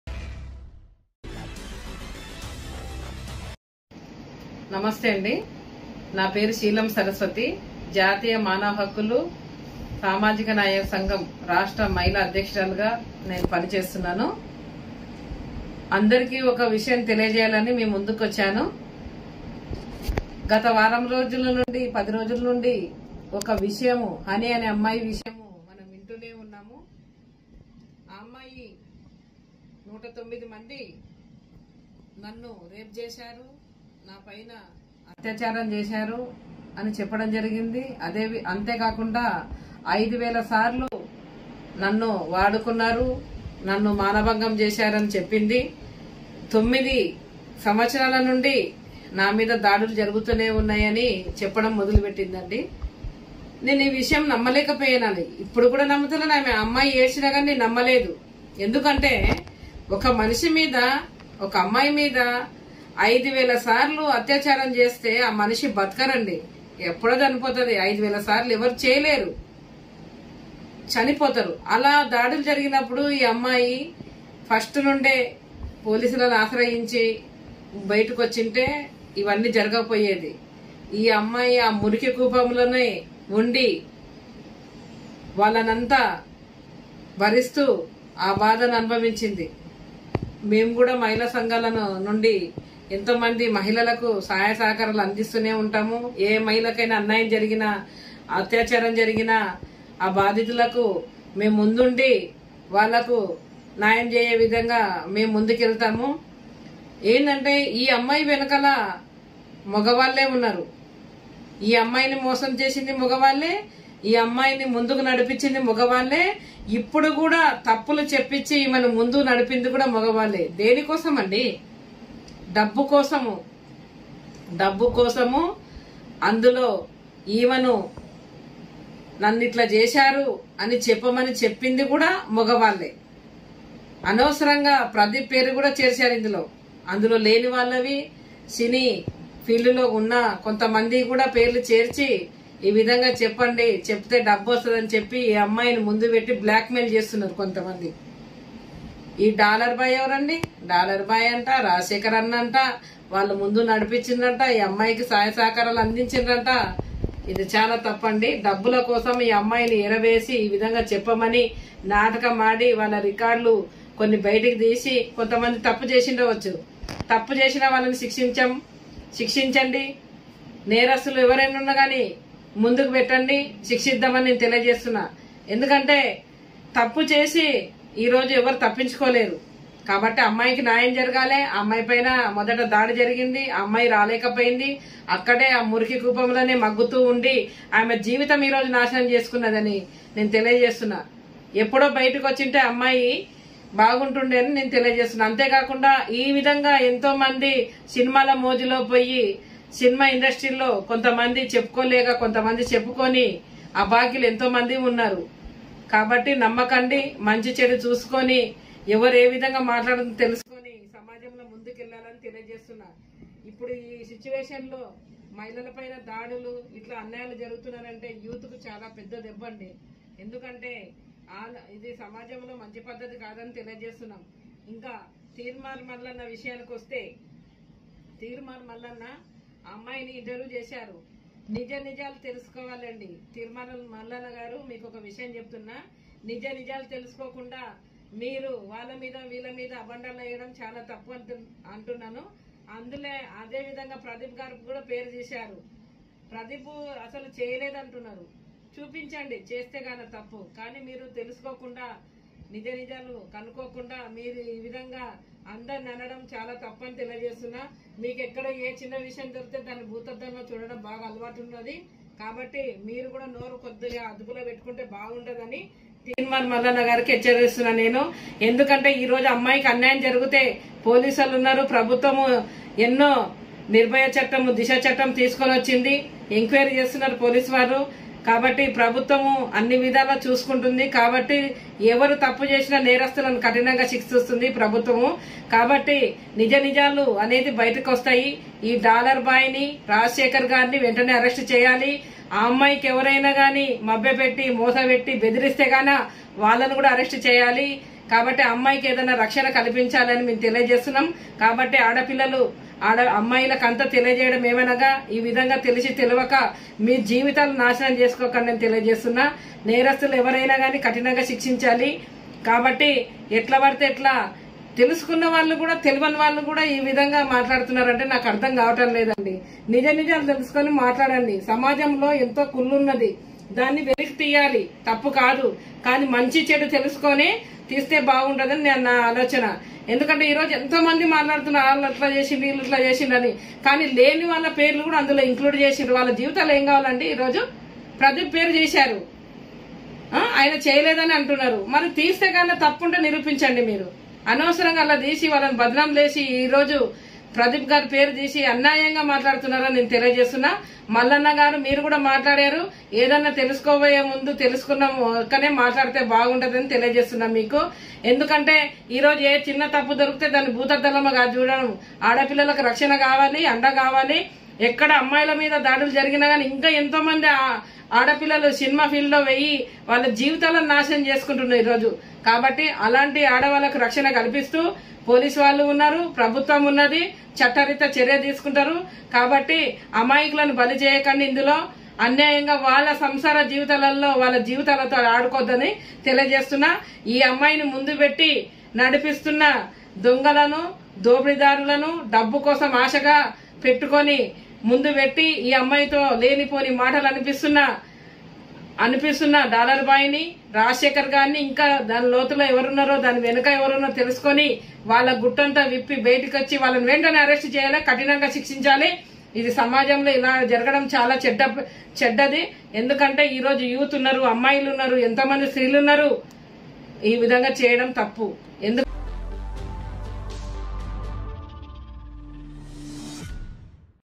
తీన్మార్ మల్లన్న ని హెచ్చరించ్చిన మహిళా అధ్యక్షురాలు || Saraswathi Sheelam || 8th Click News
తీన్మార్ మల్లన ని హెచ్చరించ్చిన మహిళా అధ్యక్షురాలు , ఆడకూతురికి న్యాయం జరగాలి - Sheelam Saraswathi